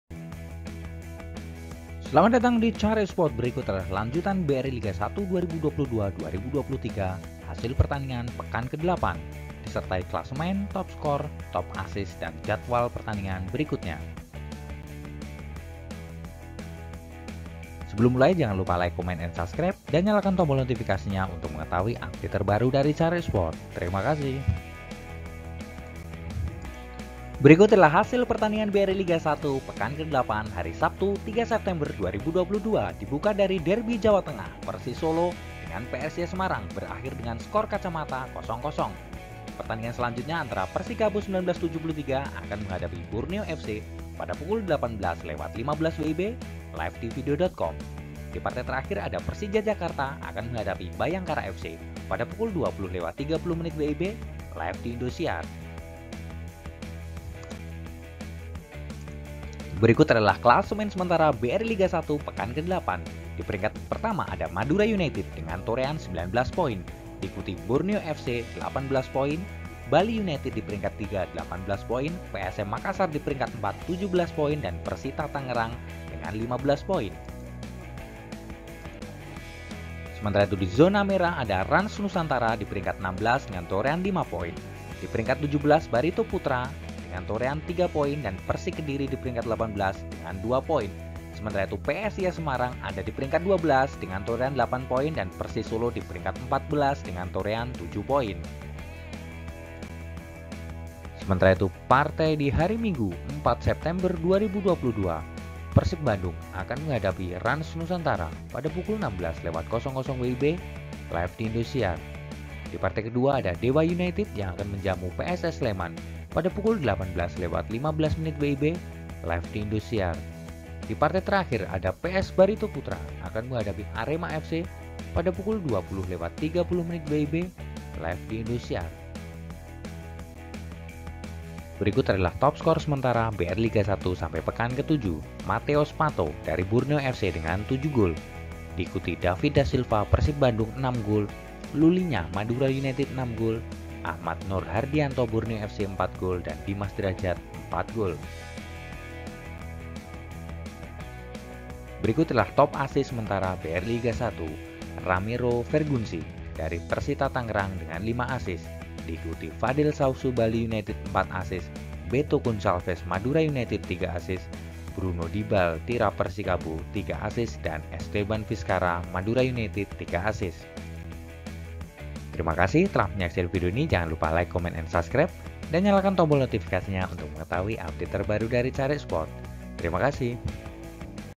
Hasil Liga 1 Hari Ini : Derbi Jateng Berakhir Skor Kacamata | Klasemen Liga 1 Terbaru
Hasil BRI Liga 1 Hari Ini
Jadwal BRI Liga 1 Hari ini
Klasemen Terbaru BRI Liga 1 Pekan 8
Top Skor Liga 1 Terbaru
Top Assist Liga 1 Terbaru

Pukul 16.00 WIB Persebaya Surabaya vs Bali United (Indosiar) 0-1
Pukul 18.15 WIB Persita Tangerang vs Madura United (Vidio.com) 0-1
Pukul 20.30 WIB Persik Kediri vs PSM Makassar (Indosiar) 0-0

Pukul 16.00 WIB Persis Solo vs PSIS Semarang (Indosiar)
Pukul 18.15 WIB Persikabo 1973 vs Borneo FC (Vidio.com)
Pukul 20.30 WIB Persija Jakarta vs Bhayangkara FC (Indosiar)